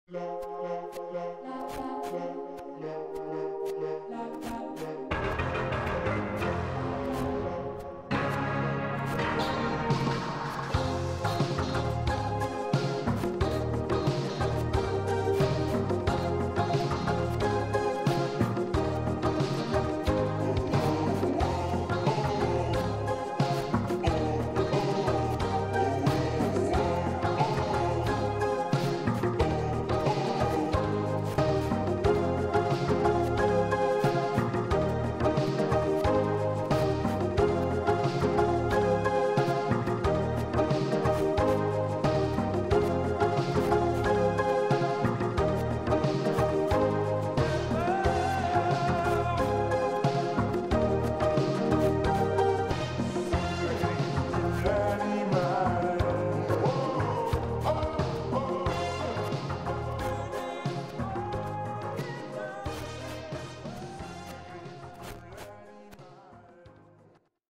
【Ｗｈｏ ｉｓ ｌｉｋｅ ｕｎｔｏ Ｇｏｄ？】vaporwave
Original photo edit and original Vaporwave composition by yours truly for Michaelmas.